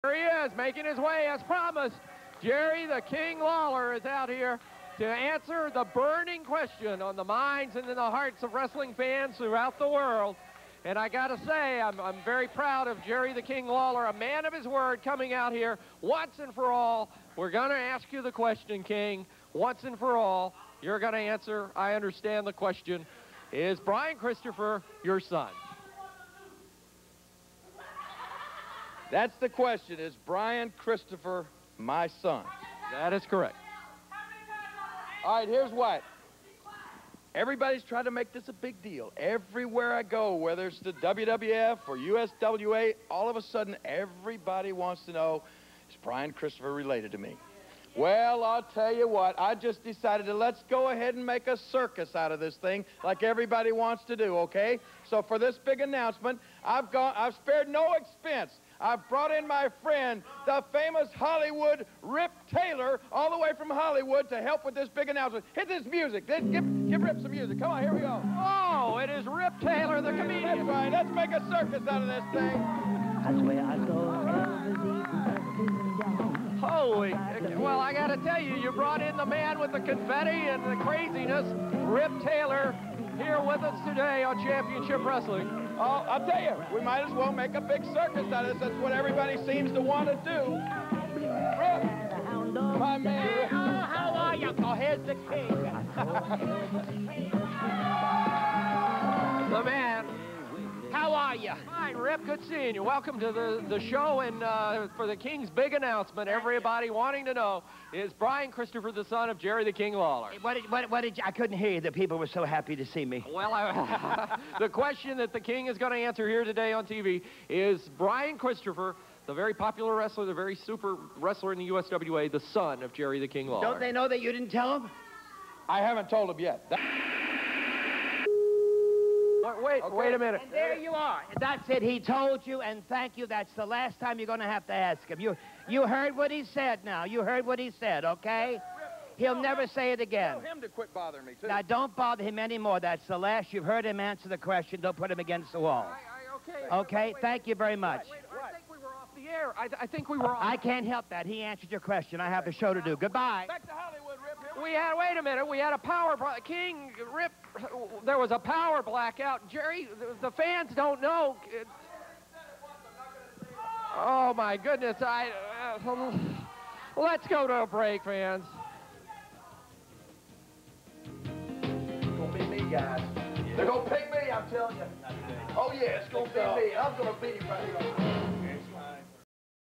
Rip Taylor in USWA
Jerry Lawler brings out Rip Taylor from Hollywood to talk about if Brian Christopher is Jerry's son or not. This is just really sad as the crowd has no idea who he is.